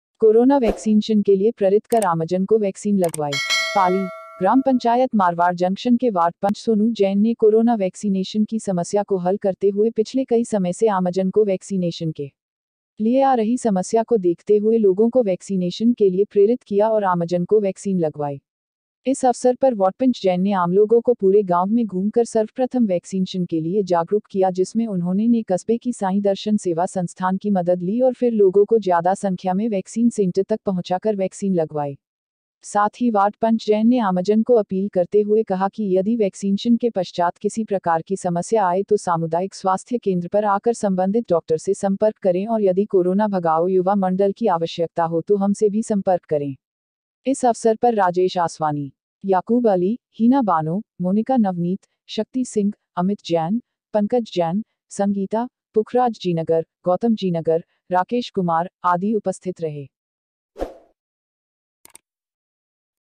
मारवाड़ जंक्शन : कोरोना वैक्सीनशन के लिए प्ररित कर आमजन को वैक्सीन लगवाई।
पाली : ग्राम पंचायत मारवाड़ जंक्शन के वार्ड पंच सोनू जैन ने कोरोना वैक्सीनेशन की समस्या को हल करते हुए पिछले कई समय से आमजन को वैक्सीनेशन के लिए आ रही समस्या को देखते हुए लोगों को वैक्सीनेशन के लिए प्रेरित किया और आमजन को वैक्सीन लगवाई। इस अवसर पर वार्डपंच जैन ने आमलोगो को पूरे गांव में घूम कर सर्वप्रथम वैक्सीनशन के लिए जागरूक किया जिसमें उन्होंने ने कस्बे की साईं दर्शन सेवा संस्थान की मदद ली और फिर लोगो को ज्यादा संख्या में वैक्सीन सेन्टर तक पहुँचा कर वैक्सीन लगवाई। साथ ही वार्ड पंच जैन ने आमजन को अपील करते हुए कहा कि यदि वैक्सीनशन के पश्चात किसी प्रकार की समस्या आए तो सामुदायिक स्वास्थ्य केंद्र पर आकर संबंधित डॉक्टर से संपर्क करें और यदि कोरोना भगाओ युवा मण्डल की आवश्यकता हो तो हम से भी संपर्क करें।  इस अवसर पर राजेश आसवानी, याकूब अली, हीना बानो,  मोनिका नवनीत, शक्ति सिंह, अमित जैन, पंकज जैन, संगीता, पुखराज जीनगर, गौतम जीनगर, राकेश कुमार, आदि उपस्थित रहे।